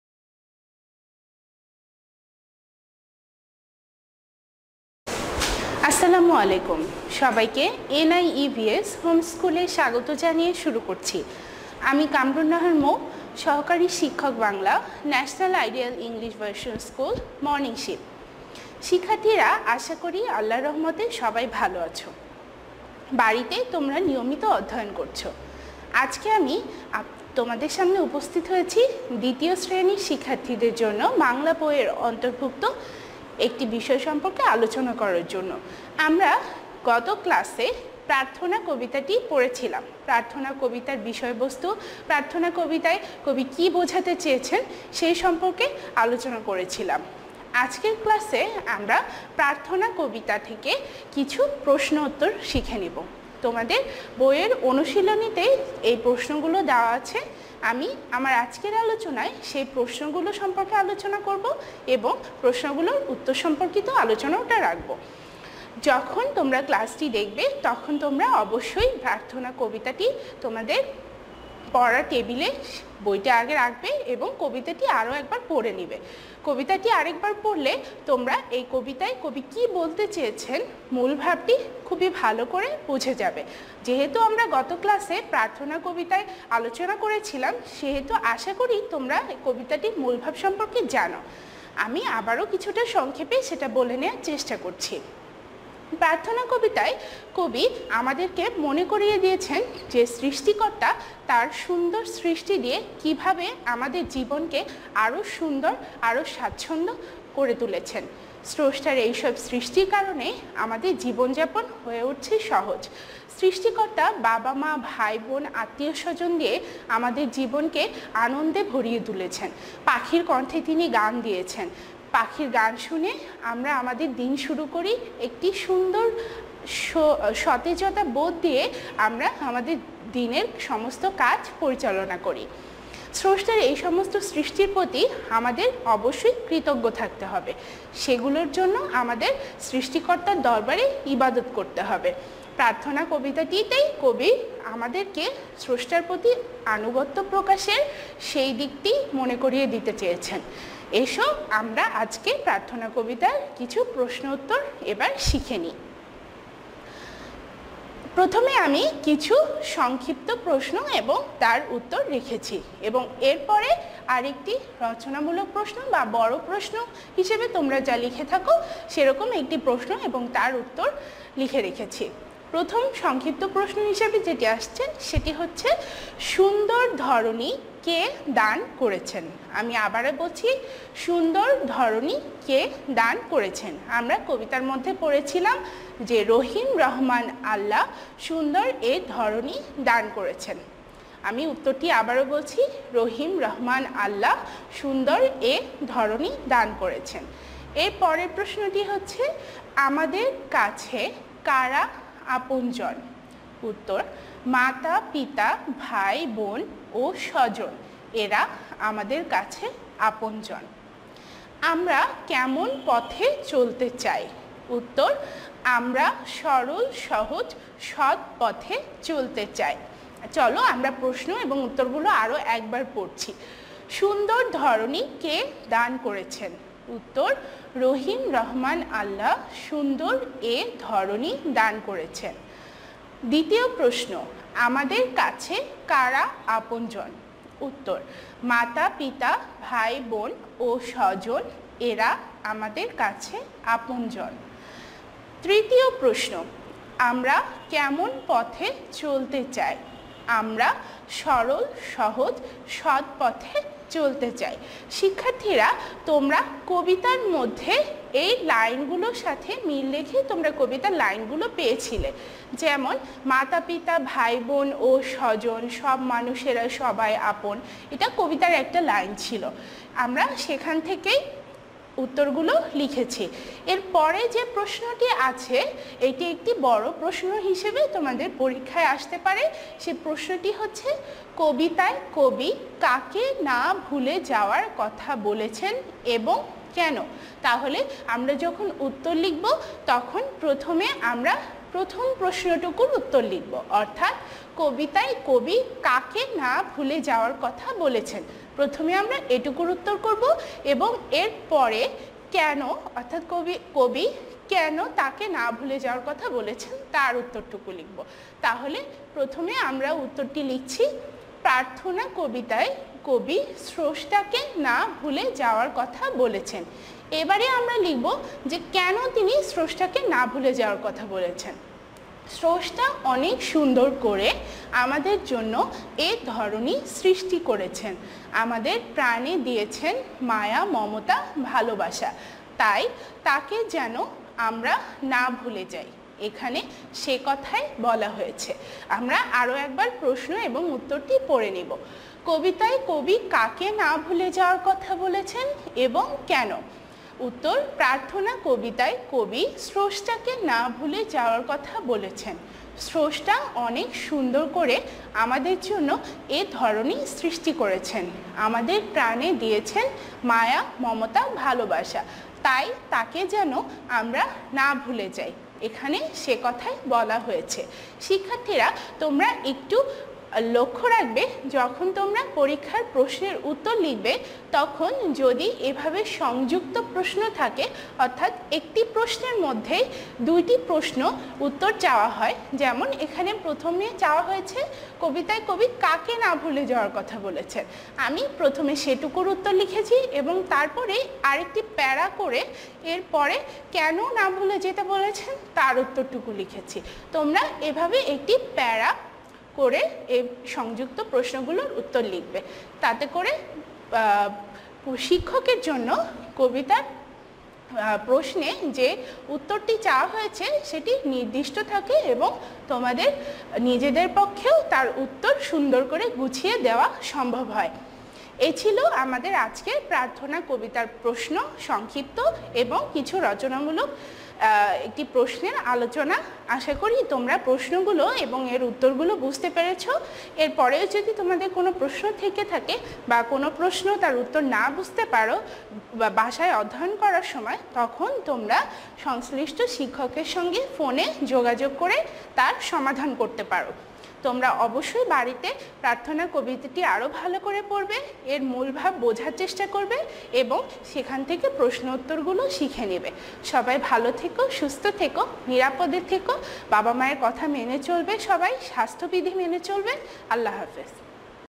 শ্রেণি: দ্বিতীয়, বিষয়: বাংলা (পাঠ-3), বিষয়বস্তু: প্রশ্ন/উত্তর [প্রার্থনা]
Annual Term
শ্রেণি: দ্বিতীয়
বিষয়: বাংলা (পাঠ-3)
বিষয়বস্তু: প্রশ্ন/উত্তর [প্রার্থনা]
শিক্ষক: কামরুন্নাহার মৌ